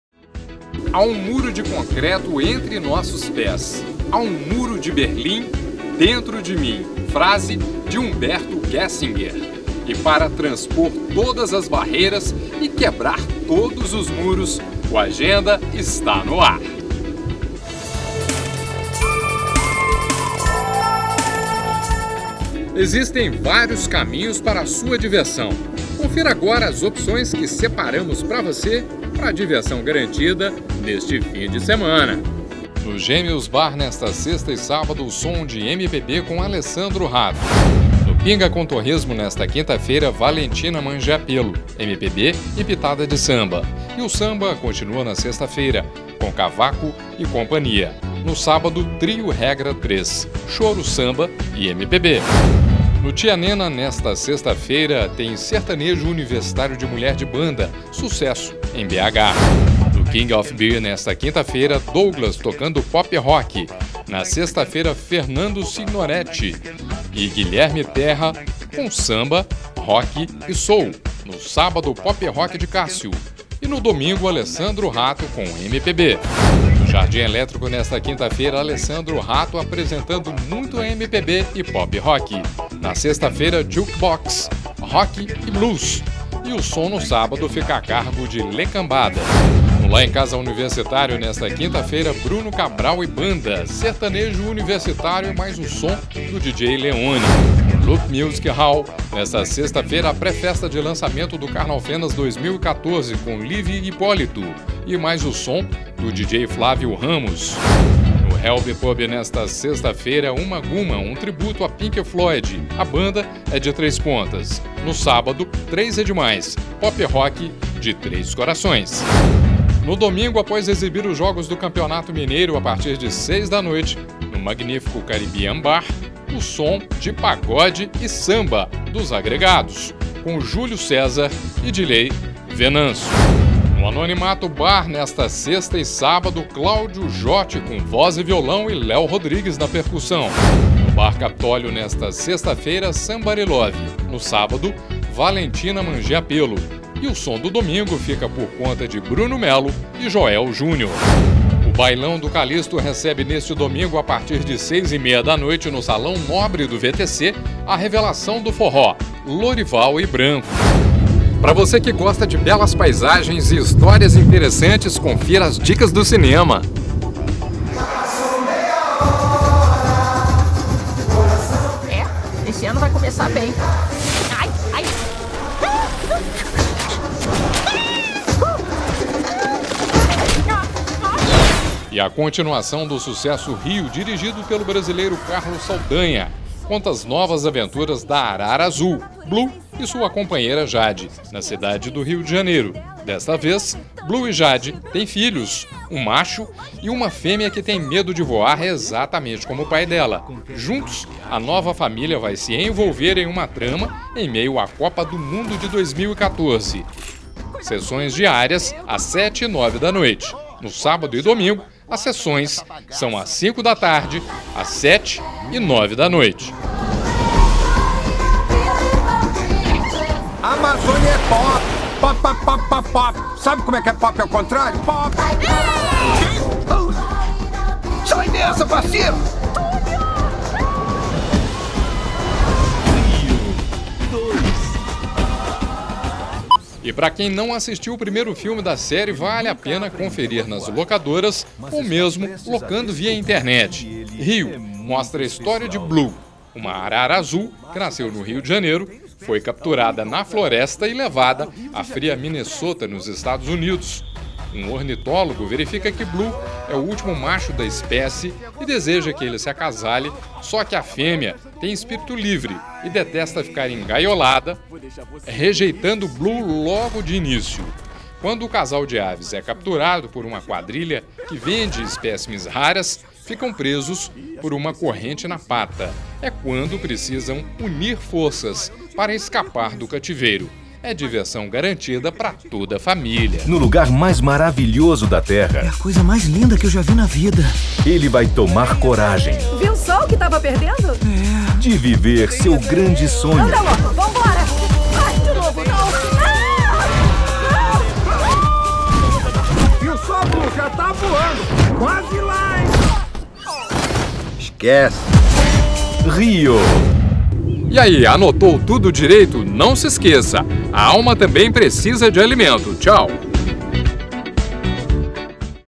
Agenda :: 27 de março a 02 de abril de 2014
Fique por dentro do cenário cultural de Varginha para os próximos dias com o repórter Euclides Oliva. (Jornal Cidade, 27 de março de 2014)